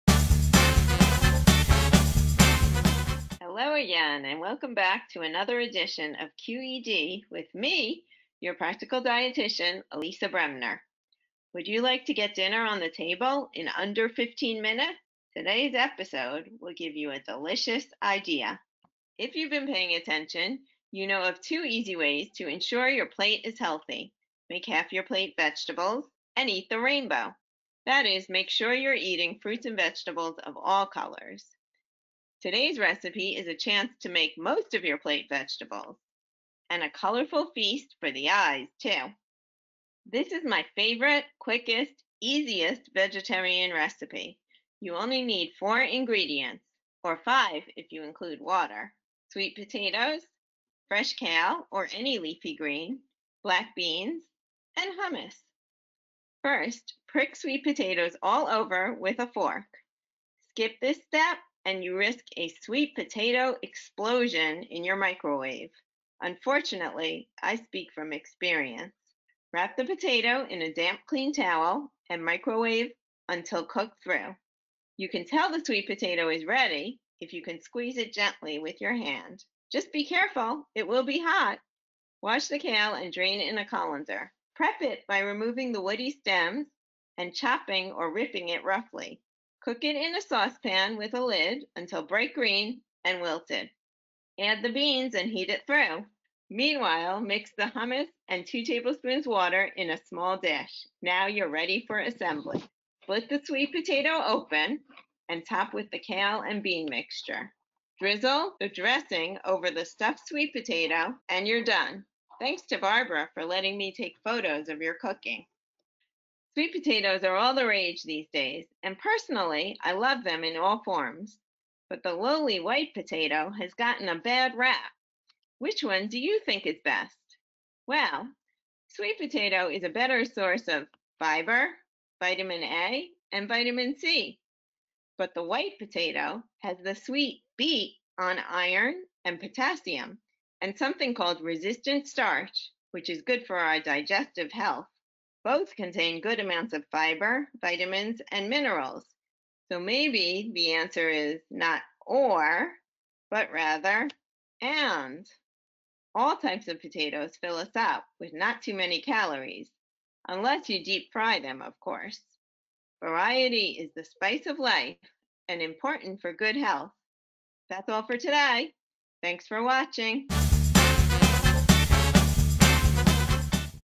Quick, Easy and Delicious! - Episode 25
Episode 25 of our Quick, Easy and Delicious series!

Join The Arc Westchester's Nutritionist - Elisa Bremner - and learn how you can make fun, healthy, and simple meals at home!